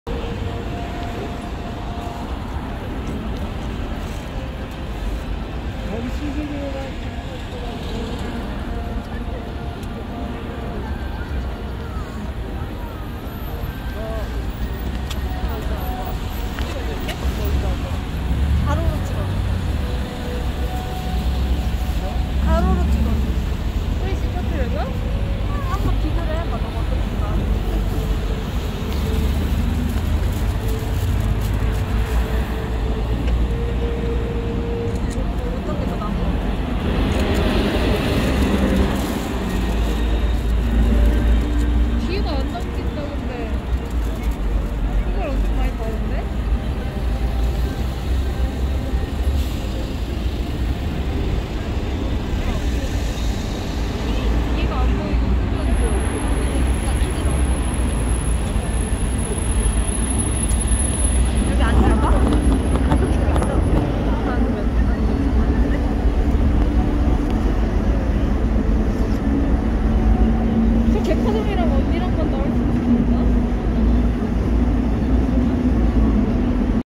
2021년 신세계백화점 크리스마스(Brilliant Christmas at Korea Department Store, Shinsegae Department Store)
2021 Shinsegae Department Store Year-end Lighting Show, Seoul, Korea Brilliant Christmas at Korea Department Store